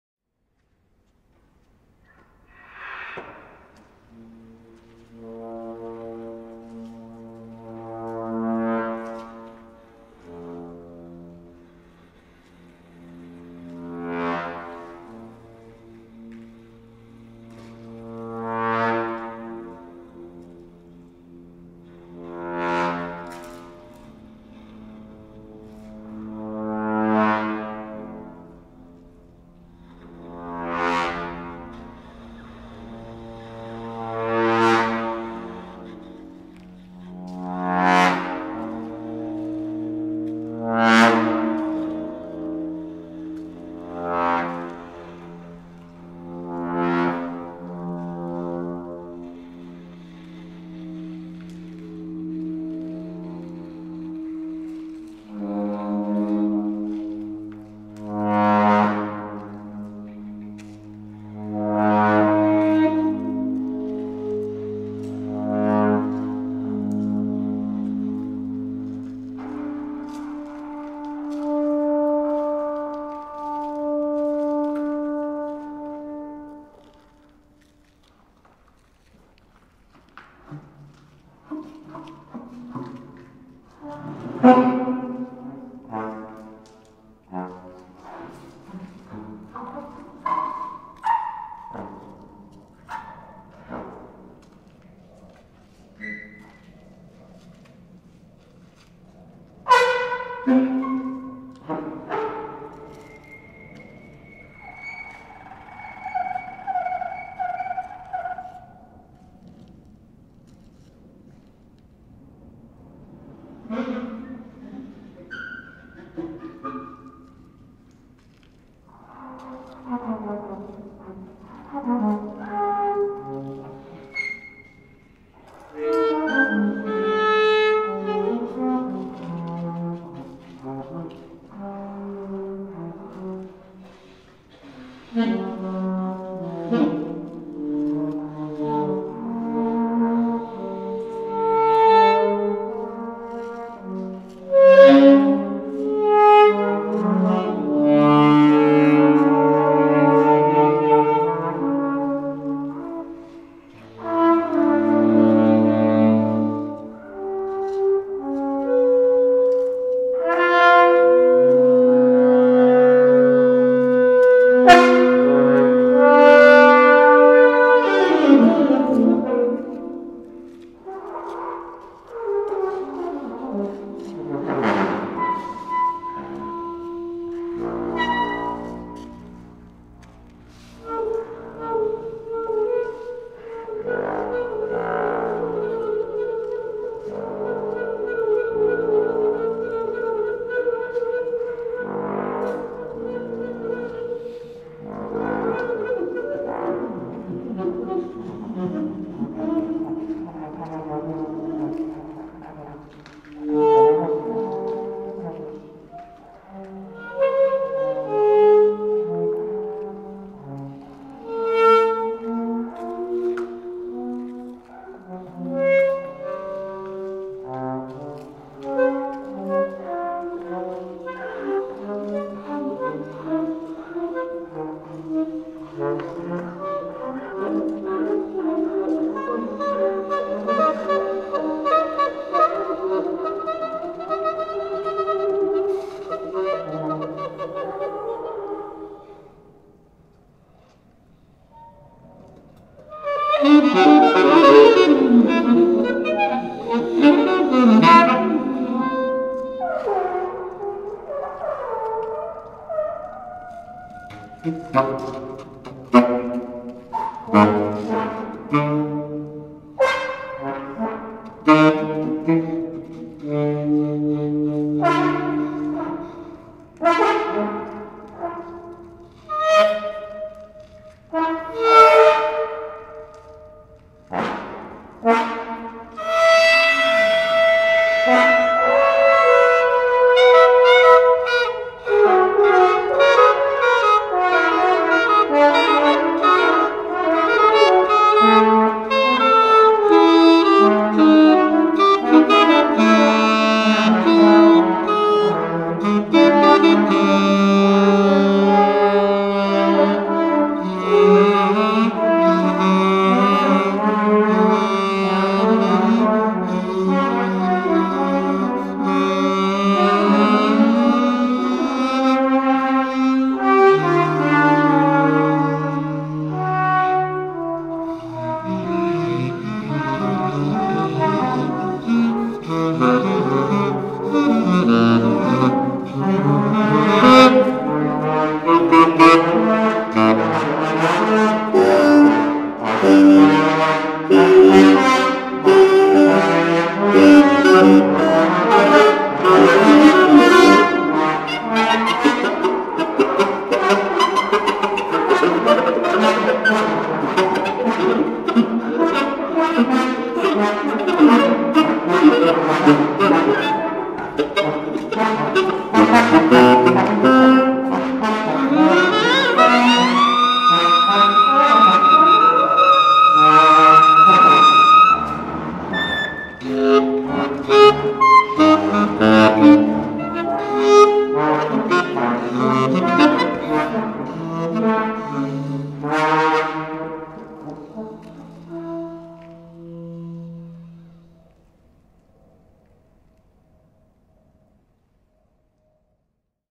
Fort Tilden Battery Harris duet part 1
Ben Gerstein, trombone; Jonathan Moritz, saxophone.